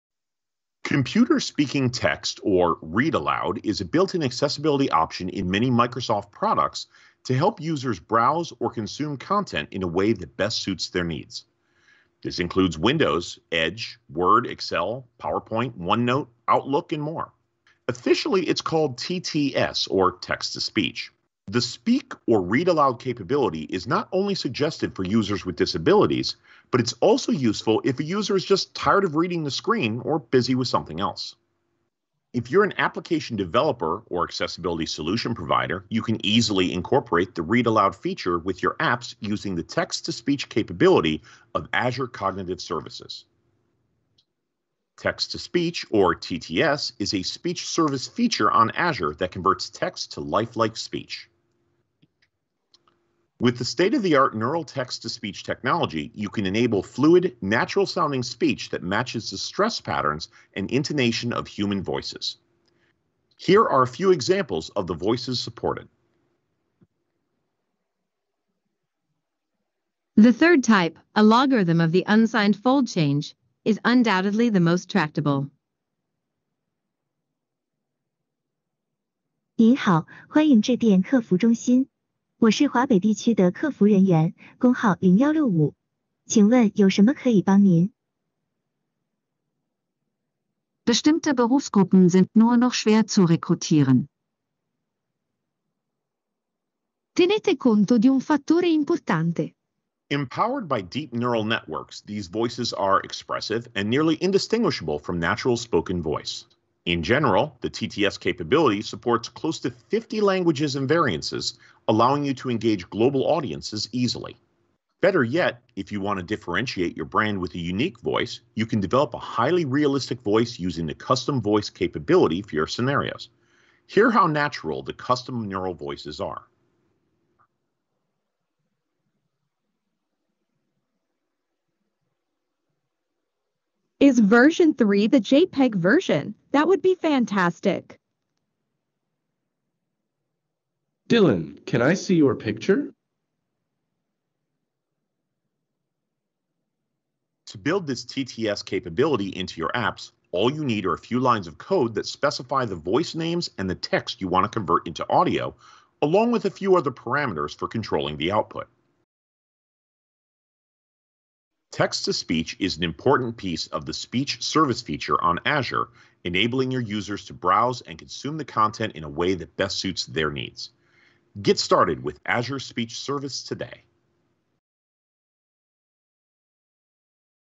Azure Text to Speech
Learn the Text-to-Speech (TTS) capability of Azure Cognitive Services in 3 minutes